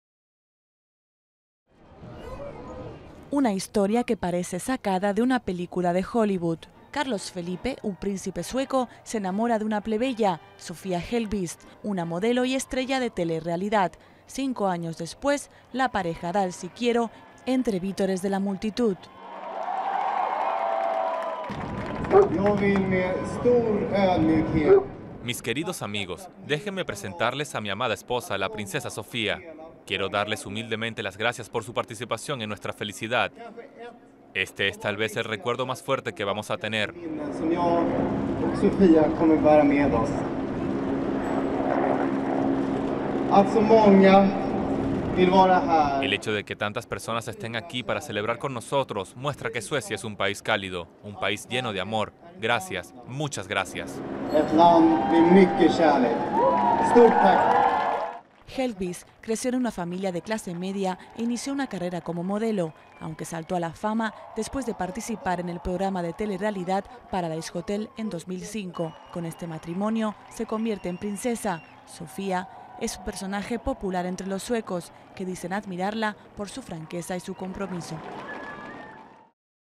Príncipe sueco se casa con estrella de telerrealidad
Centenares de suecos celebraron este sábado el matrimonio entre el príncipe Carlos Felipe de Suecia y la que fuera estrella de la telerrealidad y modelo, Sofía Hellqvist.